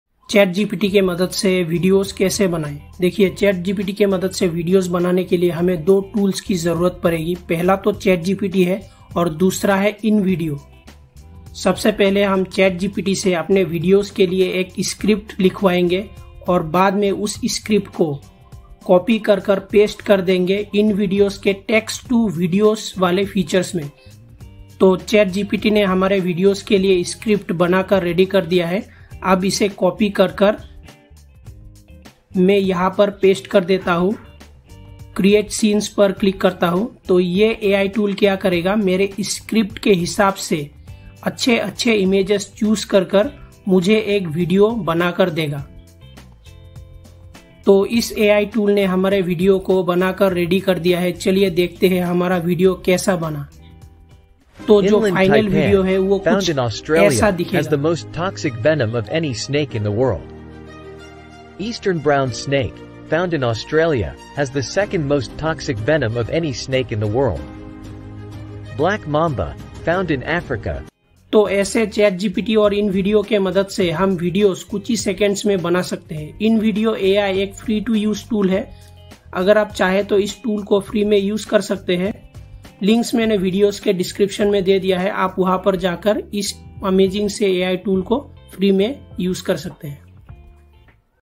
How to use ChatGPT and Invideo for video creation
TRY InVIDEO For Free

Discover the incredible power of combining ChatGPT and InVideo to revolutionize your video creation process! In this tutorial, we'll walk you through the seamless integration of these two AI tools, demonstrating how they can save you time and effort while producing high-quality content. Learn how to generate engaging scripts with ChatGPT and transform them into stunning visuals using InVideo's intuitive platform. Say goodbye to tedious editing and hello to rapid, professional video production! Don't miss out on this game-changing workflow – watch now and unlock your full content creation potential!

0:00 ChatGPT and InVideo
0:11 Generating Script With ChatGPT For Video
0:31 Pasting the script in InVideo Text To Video
0:54 Final Video Generated
1:10 Conclusion